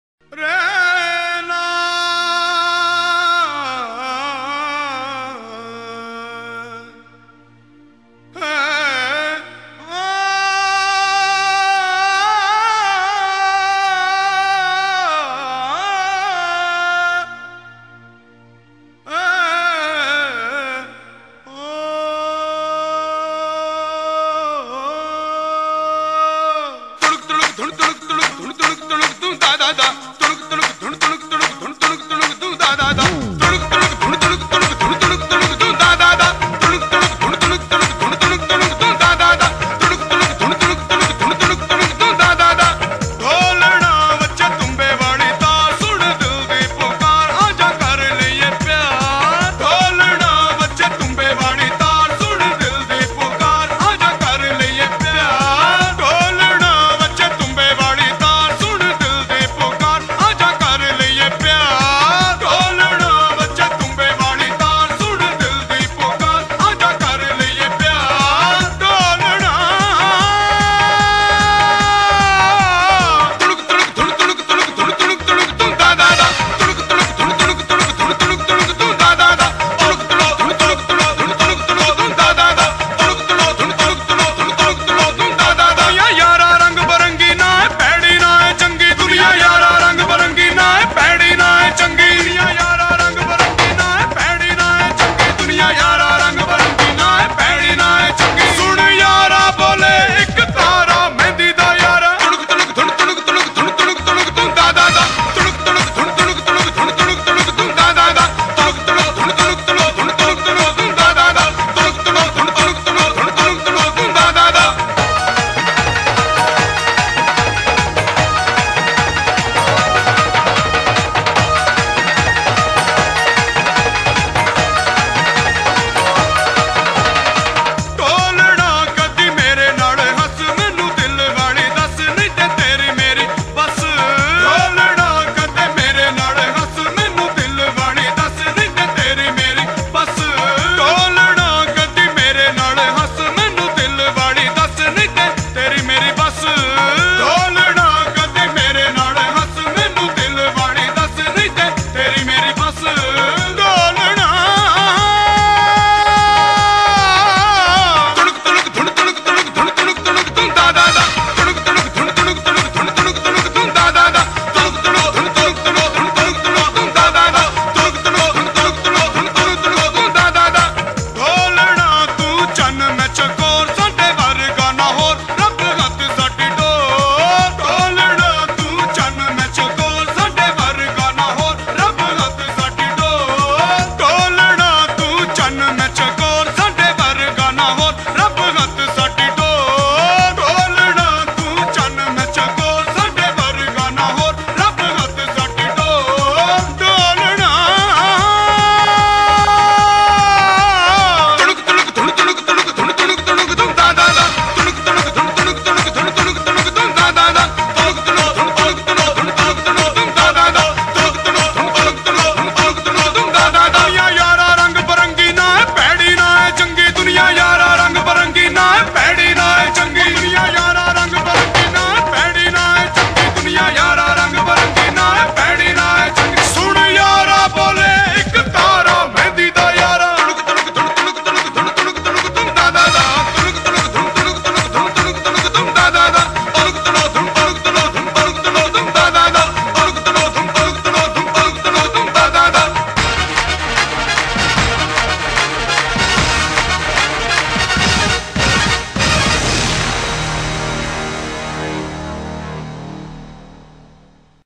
CHIYO-CHICHI DANCES WHILE I PLAY UNFITTING MUSIC
description says what?